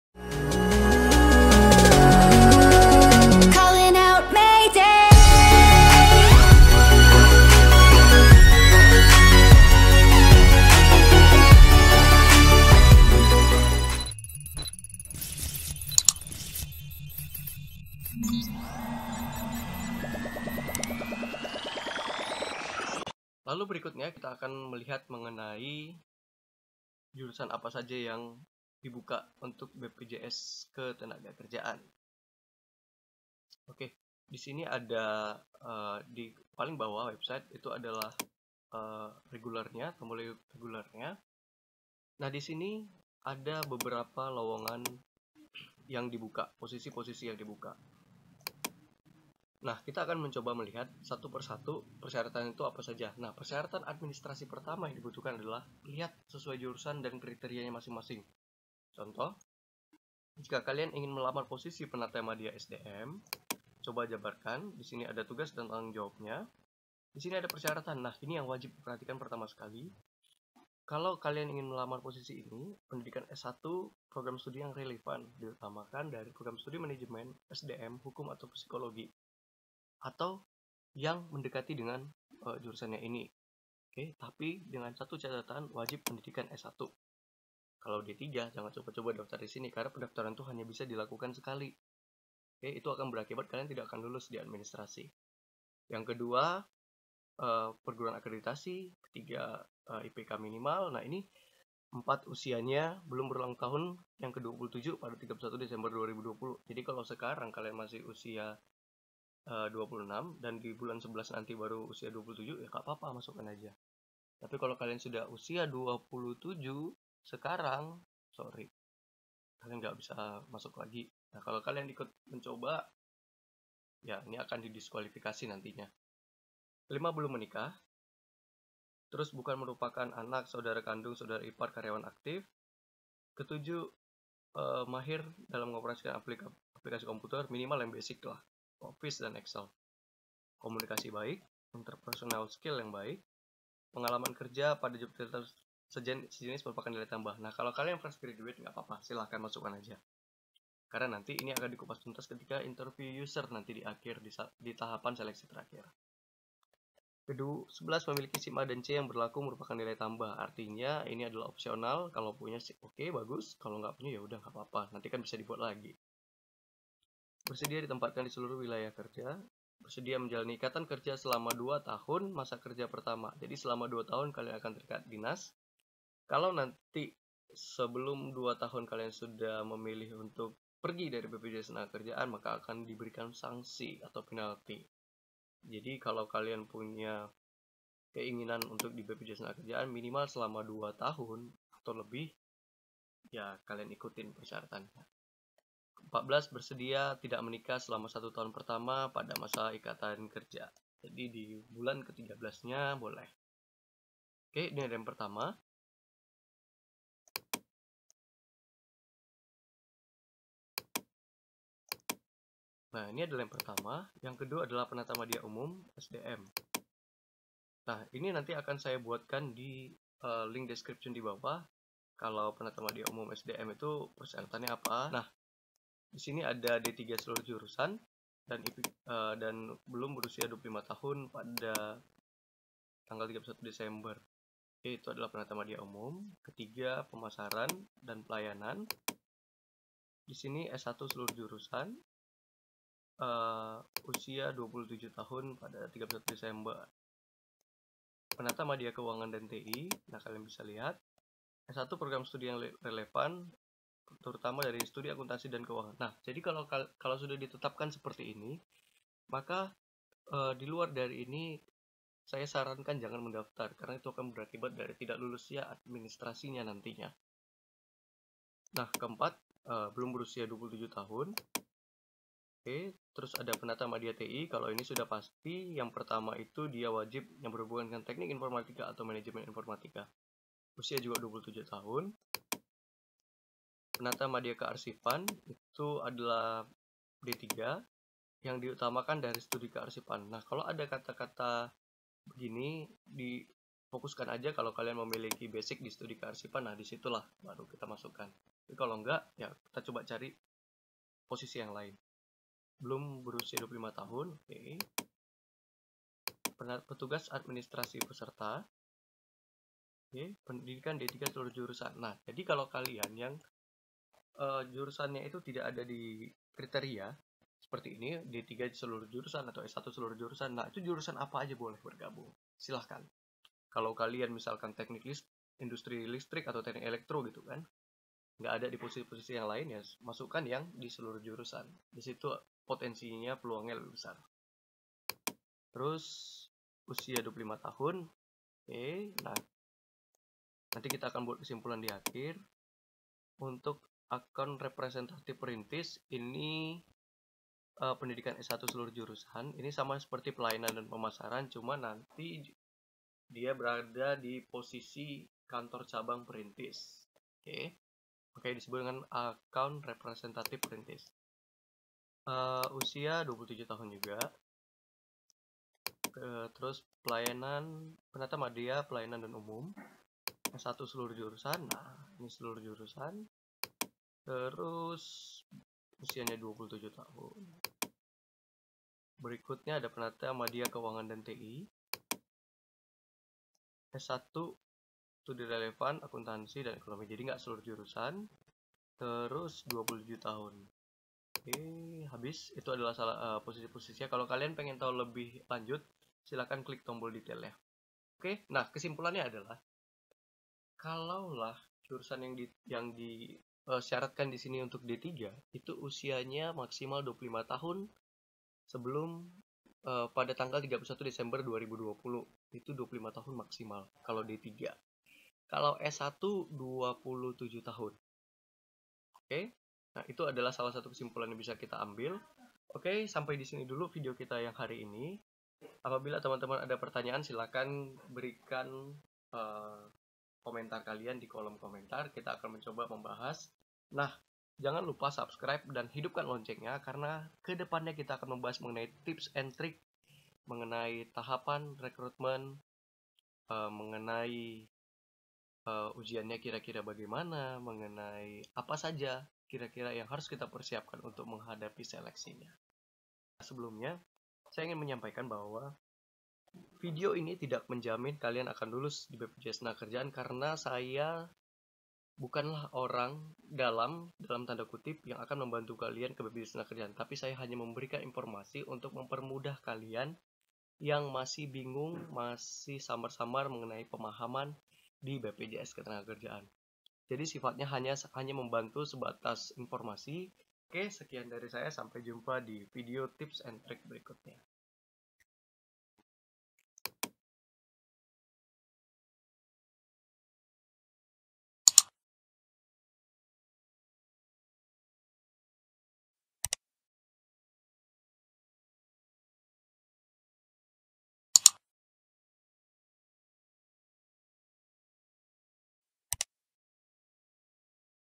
Persyaratan Rekrutmen BPJAMSOSTEK (BPJS Ketenagakerjaan)
Persyaratan Umum :
  - Belum menikah
  - Bukan merupakan Anak, Saudara Kandung, Saudara Ipar dari Karyawan
    Aktif BPJS Ketenagakerjaan
  - Mahir dalam mengoperasikan aplikasi komputer terutama MS Office dan
    MS Excel
  - Memiliki kemampuan komunikasi yang baik
  - Memiliki interpersonal skill yang baik
  - Pengalaman kerja pada job title sejenis merupakan nilai tambah
  - Memiliki SIM A dan C yang masih berlaku merupakan nilai tambah
  - Bersedia ditempatkan di seluruh wilayah kerja BPJS Ketenagakerjaan
  - Bersedia menjalani ikatan kerja selama 2 tahun masa kerja pertama
  - Bersedia tidak menikah selama 1 tahun pertama masa ikatan kerja

Persyaratan Khusus :
1. Persyaratan Penata Madya SDM :
  - Pendidikan S1 program studi yang relevan, diutamakan dari program
    studi Manajemen SDM, Hukum, atau Psikologi
  - Berasal dari perguruan tinggi yang terakreditasi minimal B
  - IPK minimal 2.75 Skala 4.00
  - Usia : Belum berulang tahun yang ke 27 pada tanggal 31 Desember 2020

2. Persyaratan Penata Madya UMUM :
  - Pendidikan D3 Seluruh Jurusan
  - Usia : Belum berulang tahun yang ke 25 pada tanggal 31 Desember 2020
  - Memiliki Kemampuan bernegosiasi

3. Persyaratan Pelayanan dan Pemasaran  :
  - Pendidikan S1 seluruh Jurusan
  - Diutamakan memiliki pengalaman kerja pada bidang pemasaran atau
    pelayanan
  - Berpenampilan menarik
  - Tinggi badan minimal :
       Pria 165 cm
       Wanita 155 cm
       Dengan berat badan proporsional
   - Memiliki kemampuan komunikasi yang baik dan jaringan yang luas
   - Memiliki passion dalam sales & marketing
   - Kemampuan berbahasa Inggris dan Mandarin merupakan nilai tambah

4. Persyaratan Penata Madya Keuangan :
    studi Akuntansi atau Keuangan
  - Mengetahui instrumen pelaporan keuangan
  - Mampu menyusun laporan keuangan
  - Mengetahui perhitungan perpajakan (PPh 21)
  - Mengetahui jenis intrument investasi
  - Mampu memonitor realisasi penggunaan anggaran
  - Mampu membuat dan menganalisa BPOP (Biaya Operasional dan
    Pendapatan Operasional)

5. Persyaratan Penata Madya TI :
 - Pendidikan S1, diutamakan dari program studi Teknik Informatika atau
      Manajemen Informatika
 - Memahami bahasa pemrograman SQL Database
 - Memahami troubleshooting terkait hardware dan software

6. Persyaratan Penata Madya Kearsipan :
 - Minimal Pendidikan D3, diutamakan dari program studi Kearsipan
 - Memiliki ketelitian dan administrasi dokumen yang baik

7. Persyaratan Petugas Administrasi Peserta :
 - Diutamakan memiliki pengalaman kerja pada bidang pemasaran dan
   administrasi

8. Persyaratan Account Representative Perintis :
    pelayanan
  - Berpenampilan menarik
  - Tinggi badan minimal :
       Pria 165 cm
       Wanita 155 cm

10. Persyaratan Penata Madya Keuangan & TI :
  - Mengetahui jenis intrumen investasi menjadi nilai tambah
  - Mampu membuat dan menganalisis BPOP (Biaya Operasional dan
    Pendapatan Operasional)